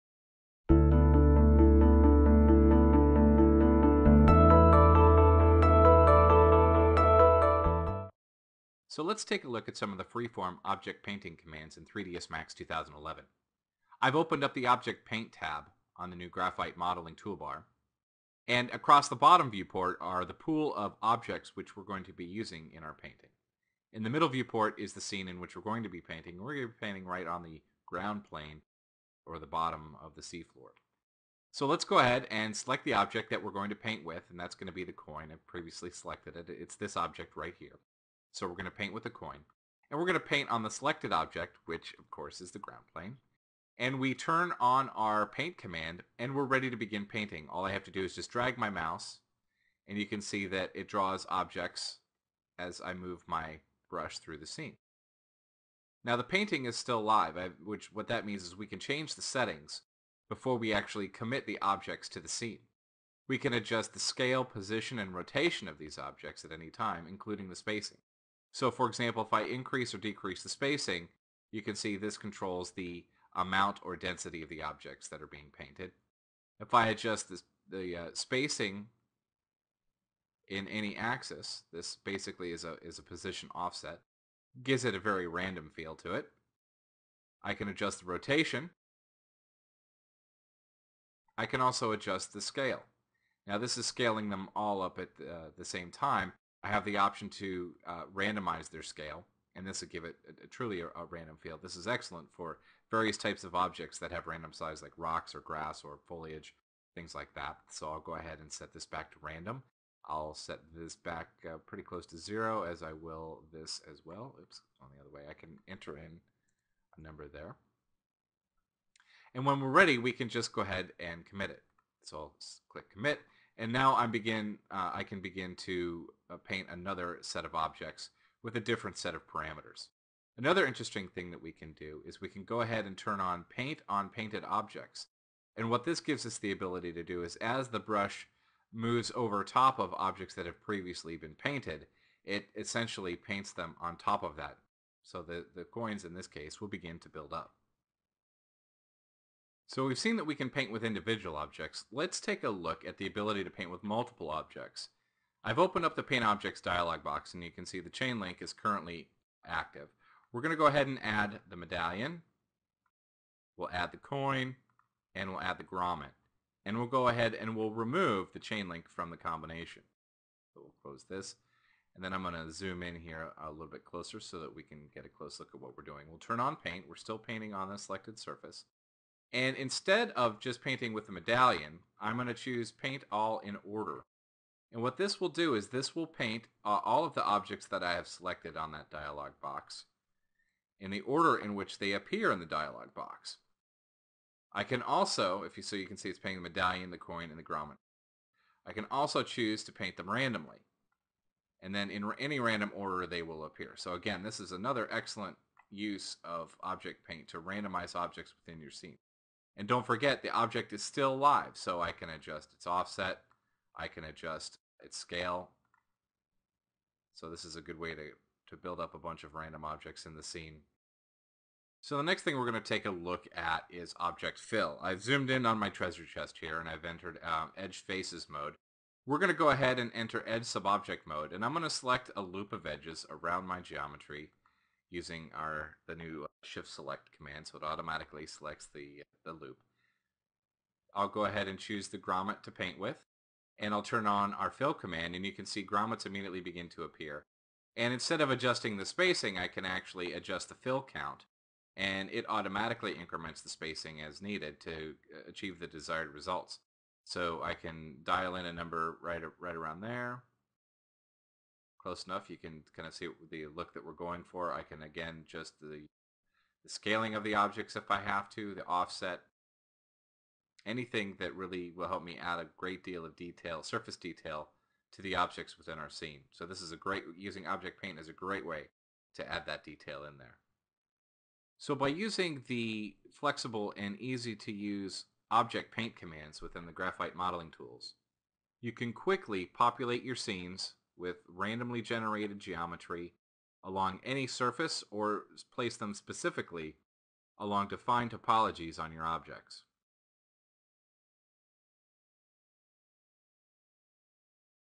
Object Paint — 3ds Max 2011 New Features
Accelerate modeling and texturing tasks with new tools that extend the Graphite modeling and Viewport Canvas toolsets: a revised toolset for 3D painting and editing textures within the viewport; the ability to paint with object brushes to create geometry within a scene; a new brush interface for editing UVW coordinates; and an interactive tool for extending edge loops.